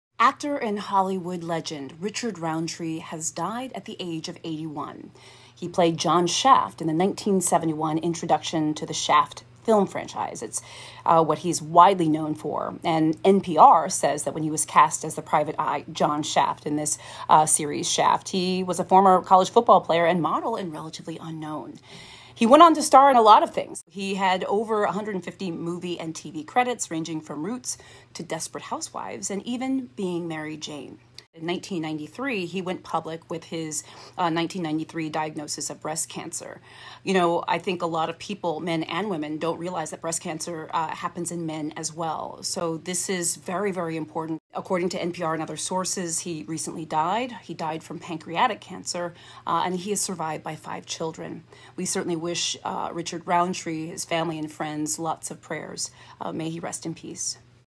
“SHAFT” Actor, RICHARD ROUNDTREE Dies at 81. Had BREAST, PANCREATIC Cancer! 🙏🏾
RIP Richard Roundtree. He Died at the age of 81 and had two different types of cancers. I discuss in this shorts video.